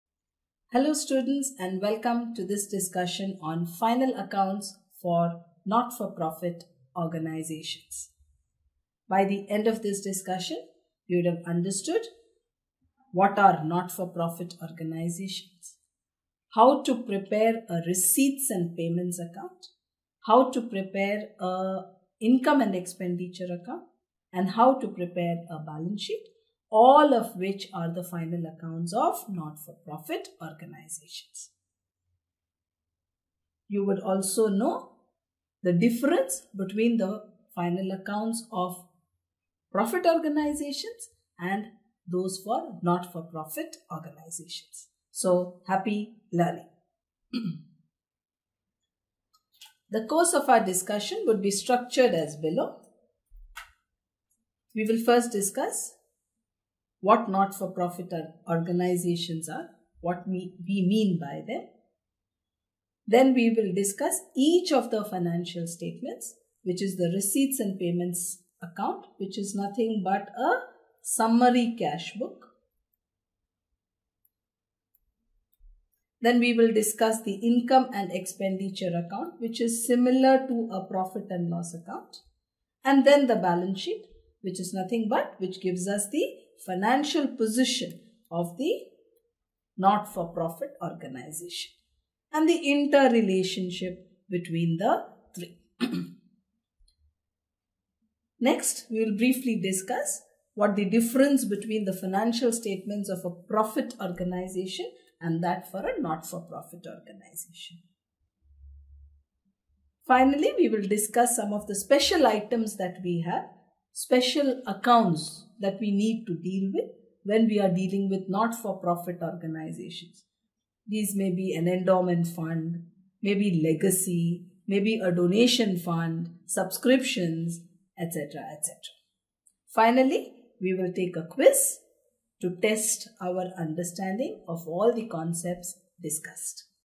Introduction NPO and it's Accounts | Non Profit Organsiation | CA CPT | CS & CMA | Class 12
Introduction NPO and it's Accounts, Learn Accounting of Not for Profit Organisation, What is NPO? What is Receipt & Payment Account, What is Income & Expenditure Account, What are Special Fund for an NPO, .